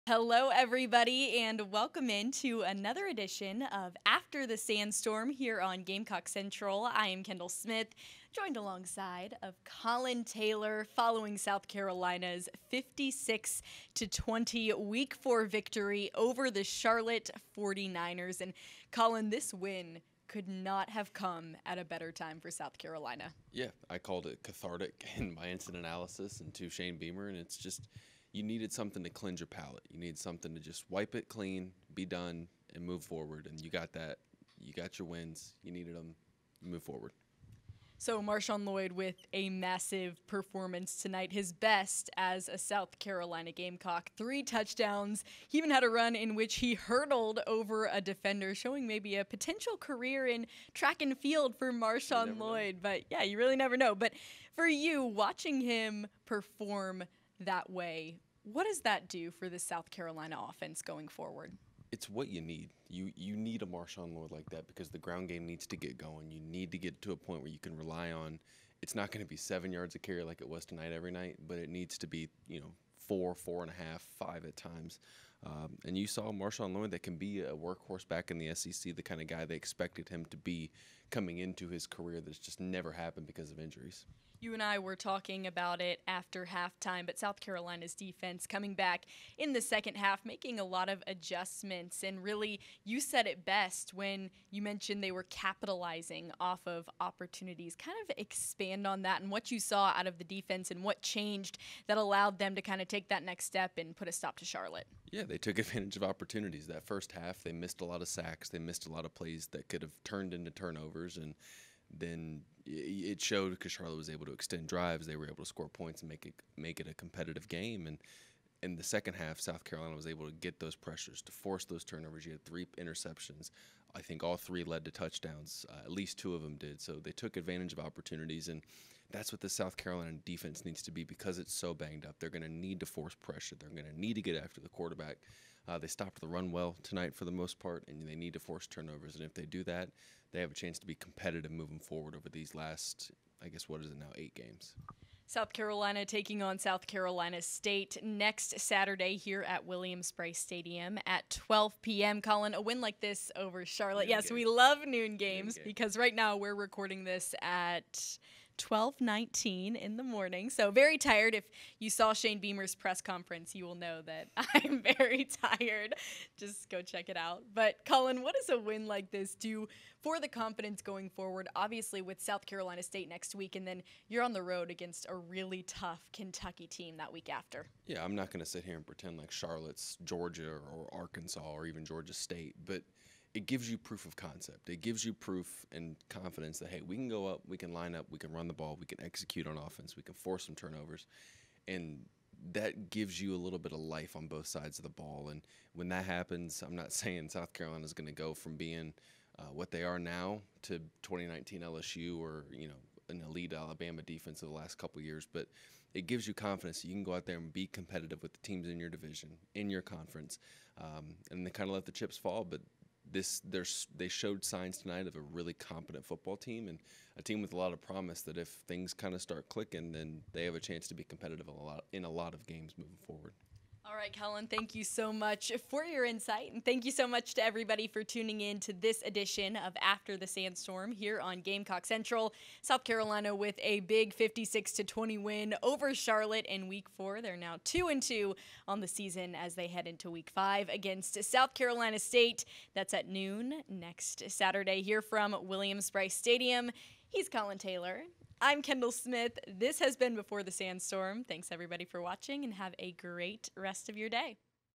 After the Sandstorm | Gamecocks win big over Charlotte
Join Collyn Taylor and Kendall Smith on After the Sandstorm, as South Carolina defeated the Charlotte 49ers 56-20 on Saturday.